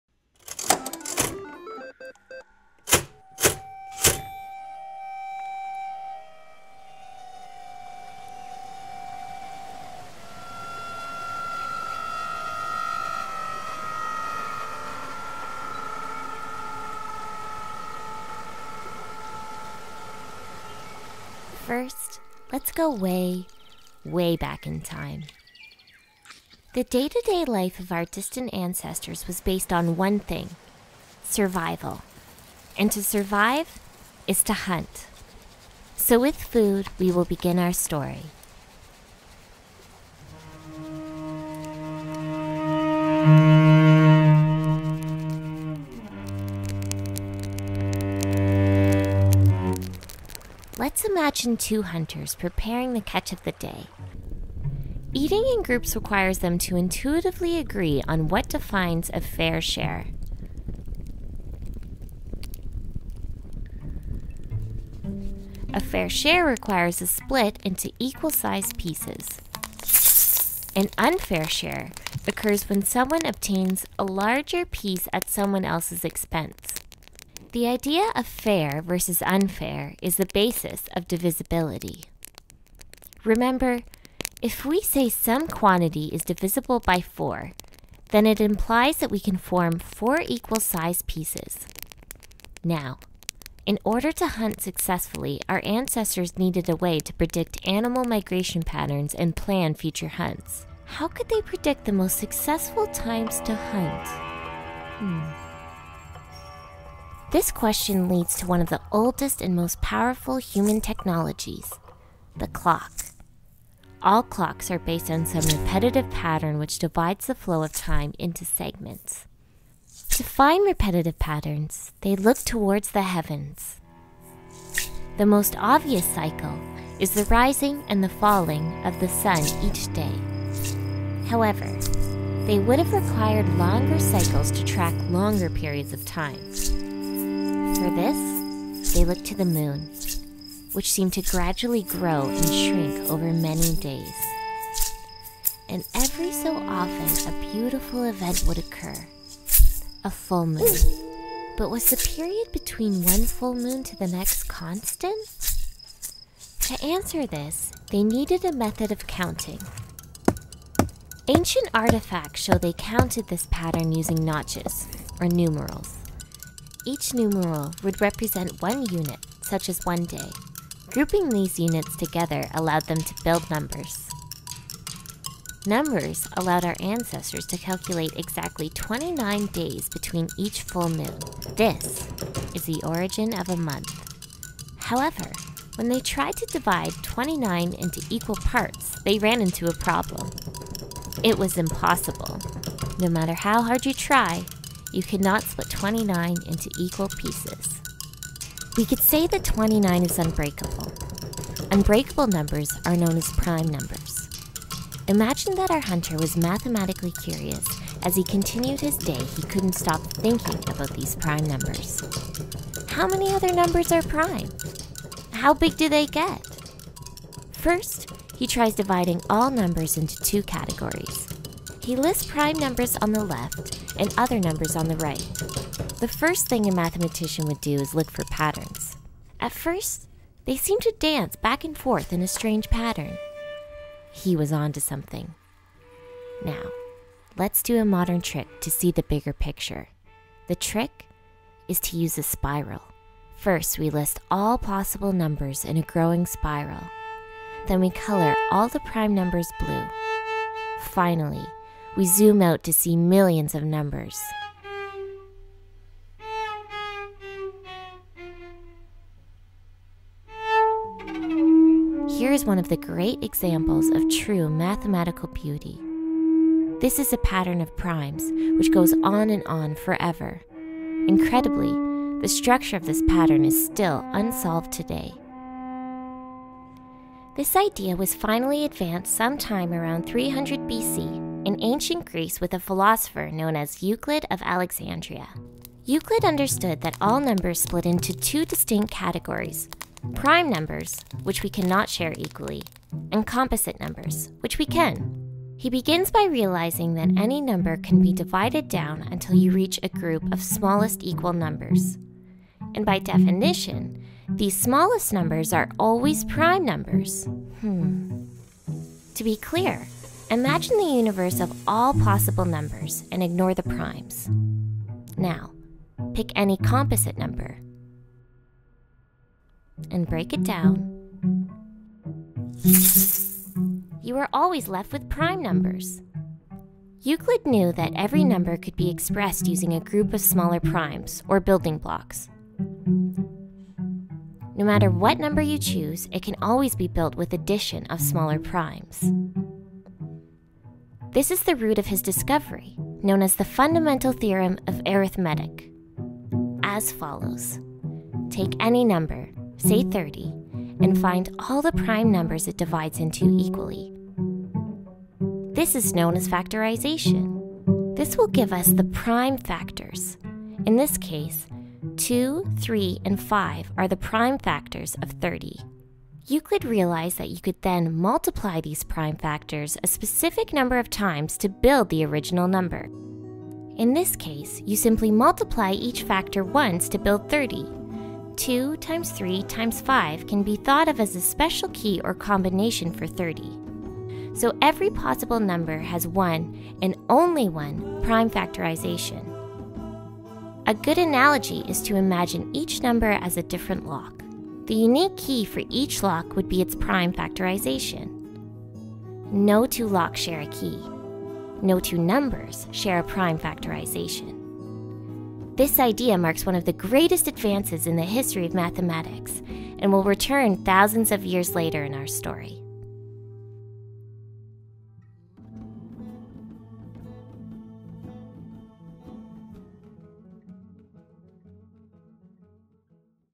Gambling with Secrets: Part 2/8 (Prime Factorization)
This chapter explores numerals, divisibility & Euclid's fundamental theorem of arithmetic (prime factorization) from a Caveman's perspective.